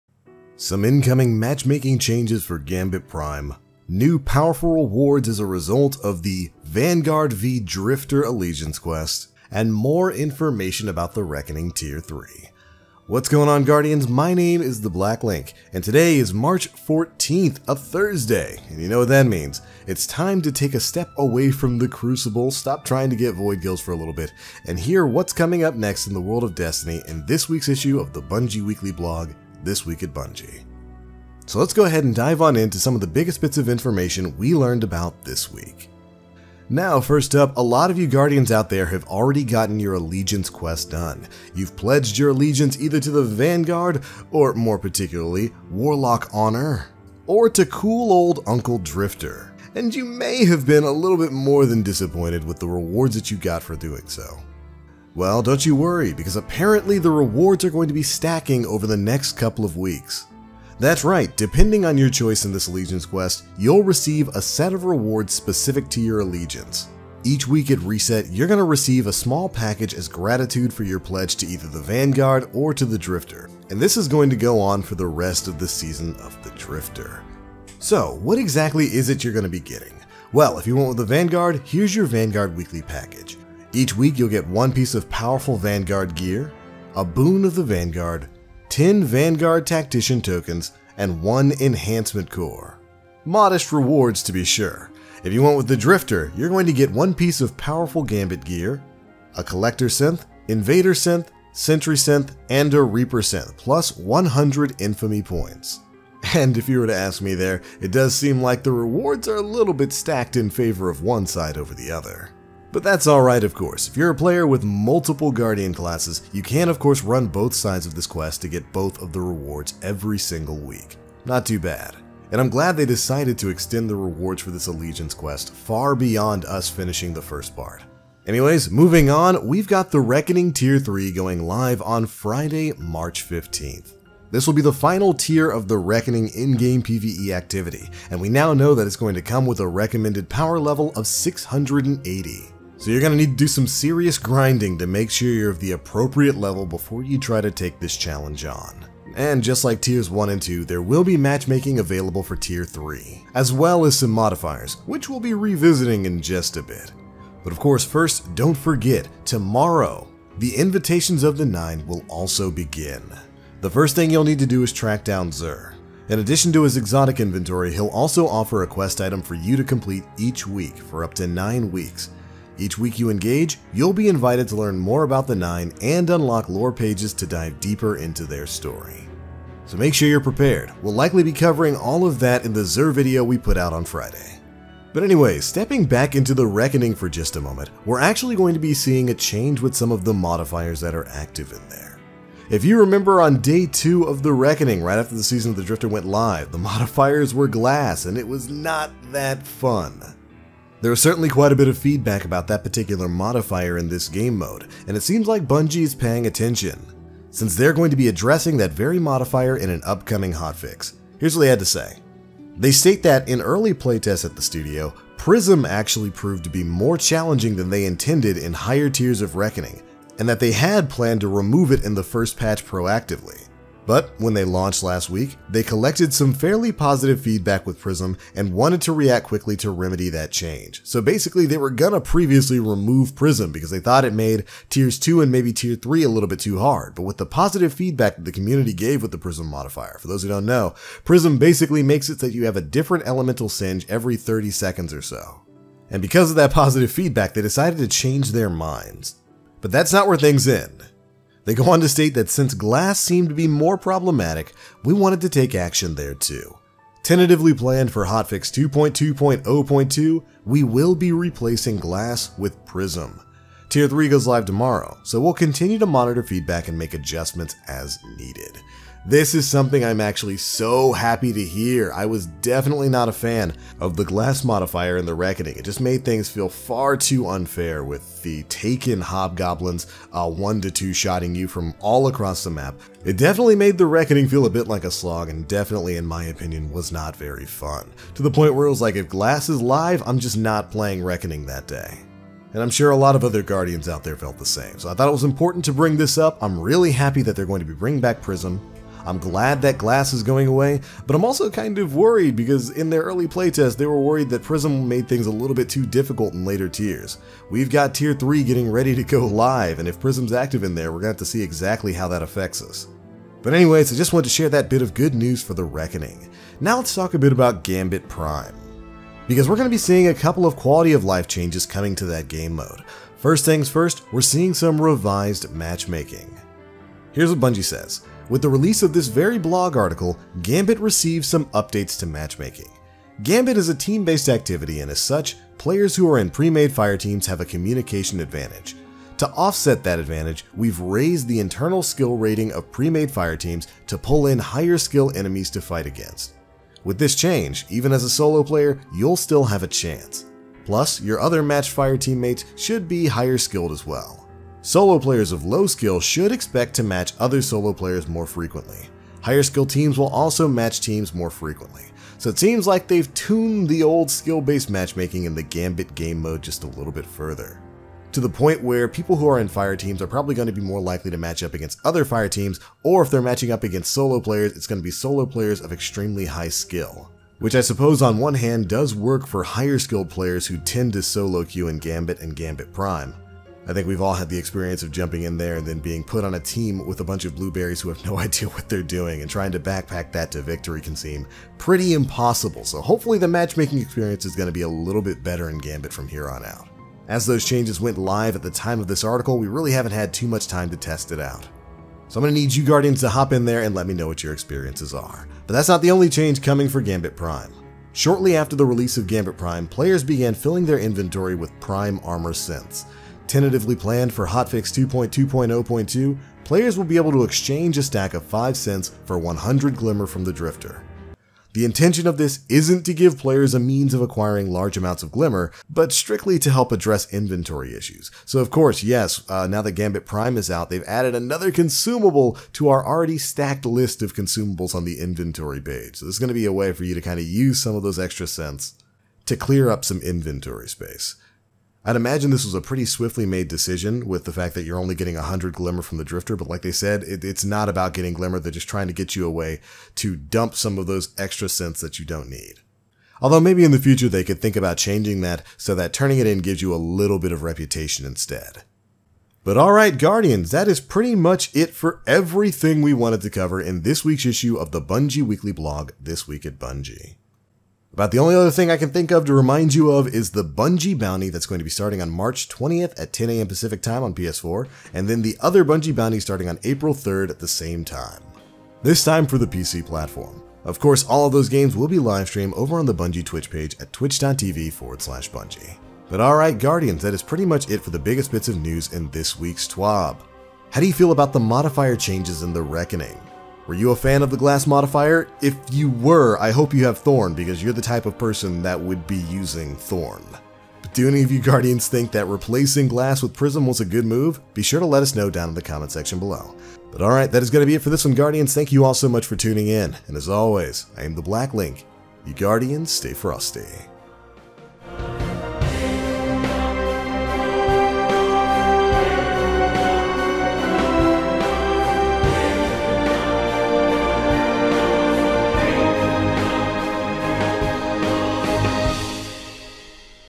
Destiny 2 | TERRIBLE Modifier REMOVED From Reckoning! Gambit Synth & Matchmaking Update!
This Week at Bungie, we learn more about some upcoming changes to the Reckoning in the Season of the Drifter for Destiny 2. The Glass modifier is being DESTROYED, and there are some Gambit QoL changes on the horizon. All that and more Destiny 2 News

Become a channel member for new, exclusive rewards!